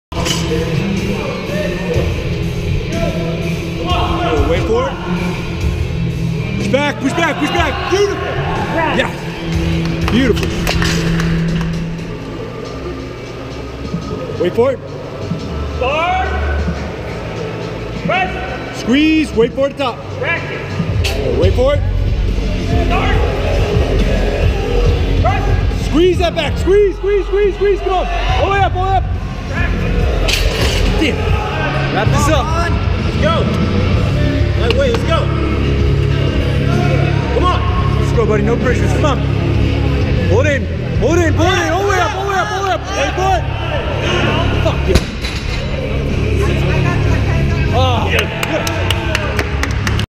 Fitz -GTS USPA Conejo Valley Open Powerlifting Competition Oct 2017
Fitz having an awesome day going 8/9! He finished with a 545 squat, 319 bench (missing 336 on a technical), 556 deadlift and a 1422 total weighing in at 204 lbs! Very impressive day!